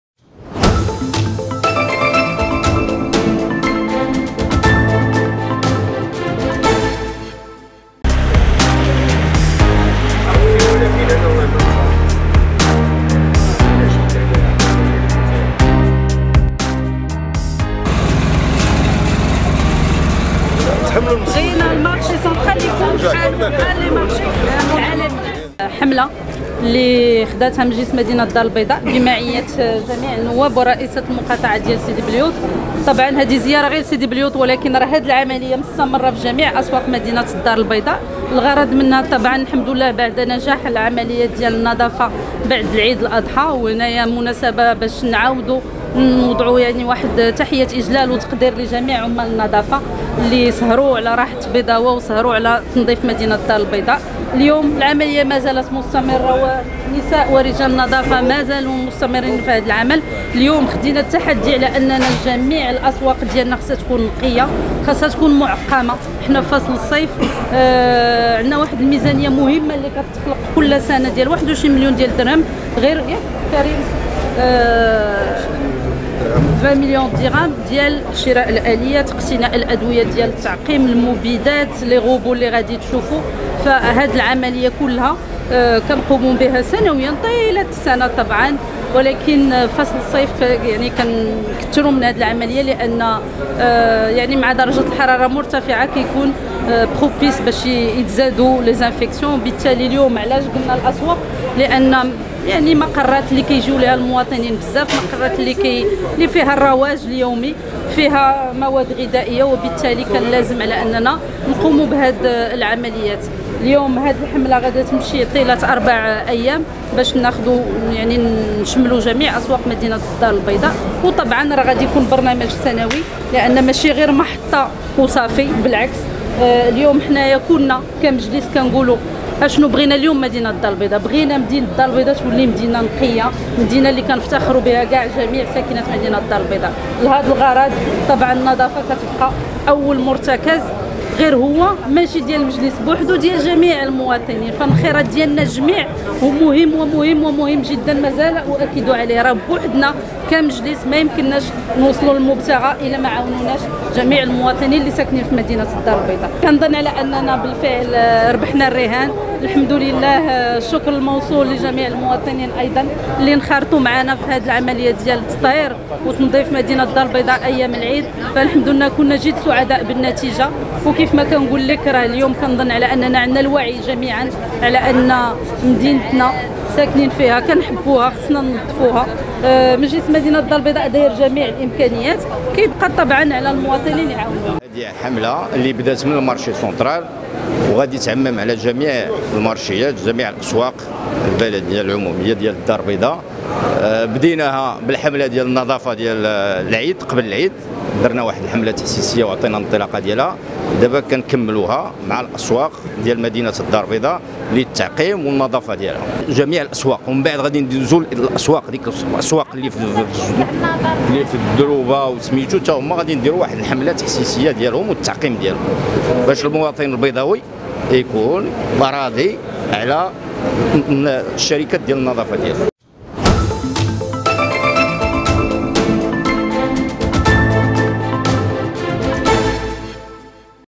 البيضاء.." كازا بيئة" تطلق حملة لتنظيف وتعقيم الأسواق الجماعية
أطلقت شركة البيضاء للبيئة تحت إشراف مجلس مدينة حملة لتنظيف وتعقيم الأسواق الجماعية بالعاصمة الاقتصادية

# أخبار_عاجلة